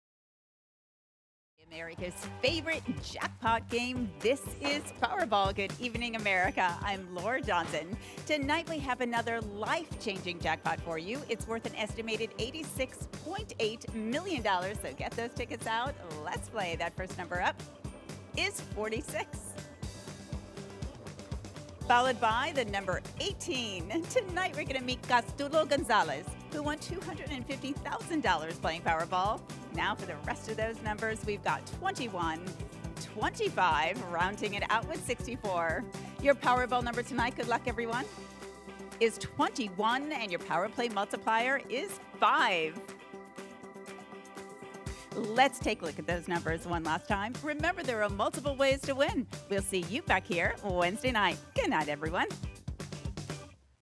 Powerball Number: $86.8 Million Jackpot, Oct. 23, 2023
Here are your winning Powerball numbers for the Oct. 23, 2023, jackpot. It is an estimated $86.8 million. What would you do with that kind of money?